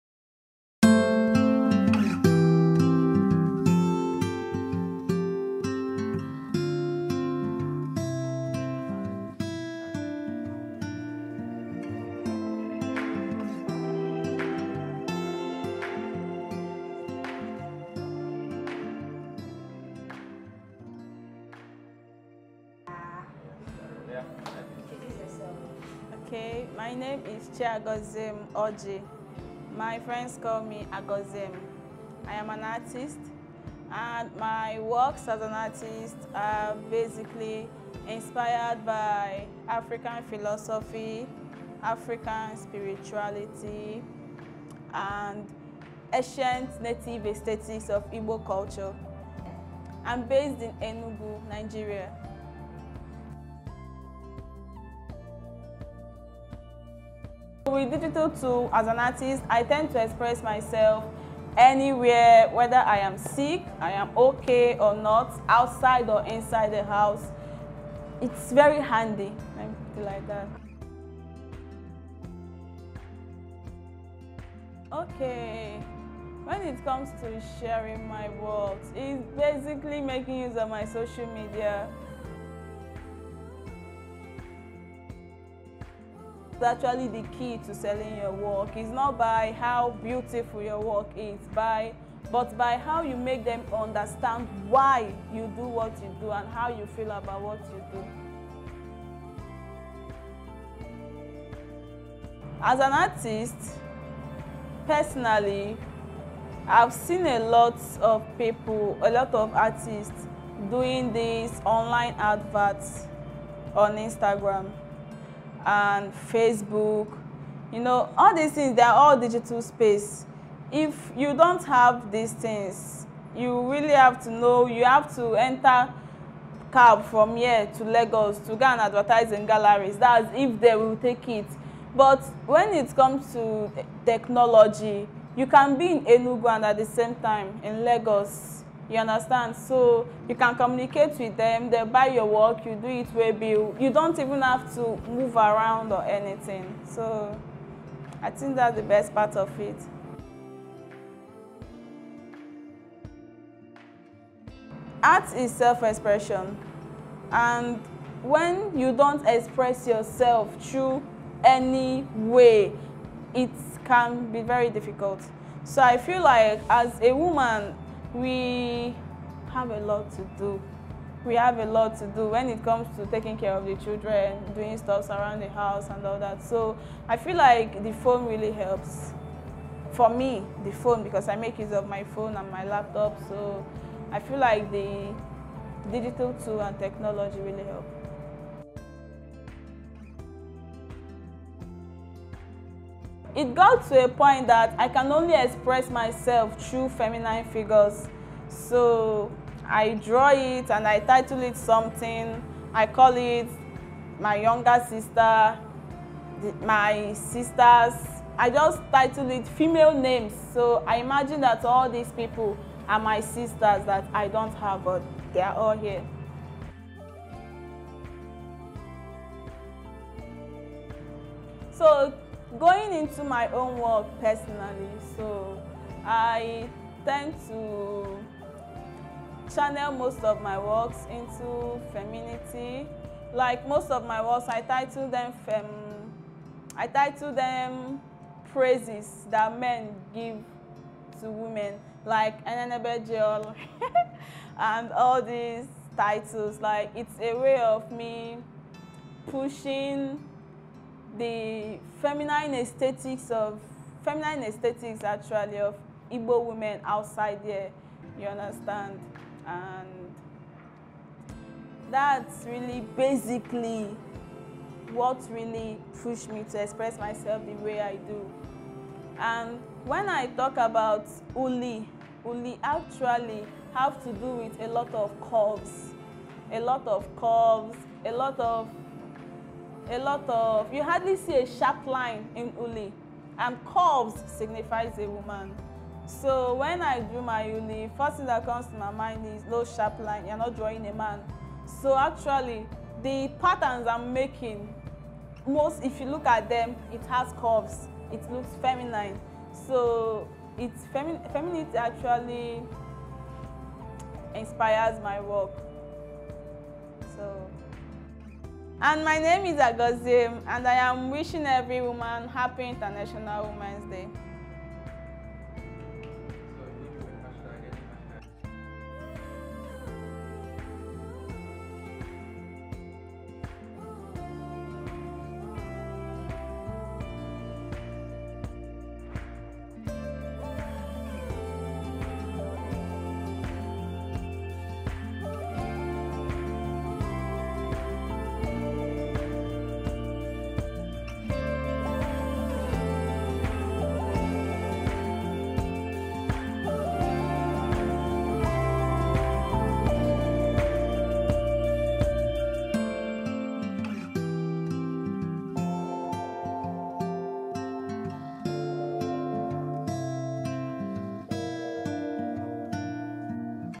A chat with Chiagoziem Orji
Chiagoziem Nneamaka Orji (Instagram: @ulitangle) is an artist, painter and pattern maker. We had a chat with her for the 2023 International Women Day Uli Art Festival.